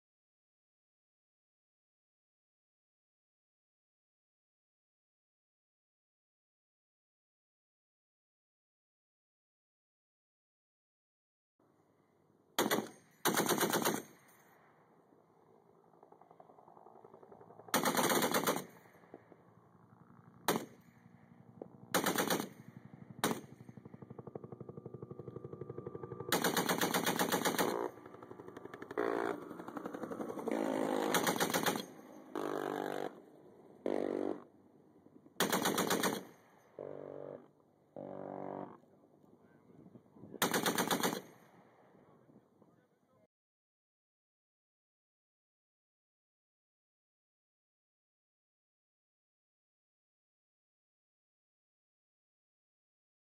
Shooting a M2 with Optical Sight
The Browning Machine Gun, Cal. .50, M2 was adopted by the United States between the world wars and has been used extensively ever since. Chambered for the the 12.7×99mm round, more commonly known as the .50 BMG, it is effective against infantry, light armored vehicles, and aircraft. In an effort to improve accuracy at extended distances for static emplaced guns, sights were developed for the M2 starting in WWII. The M1 Telescopic Sight is and example of this.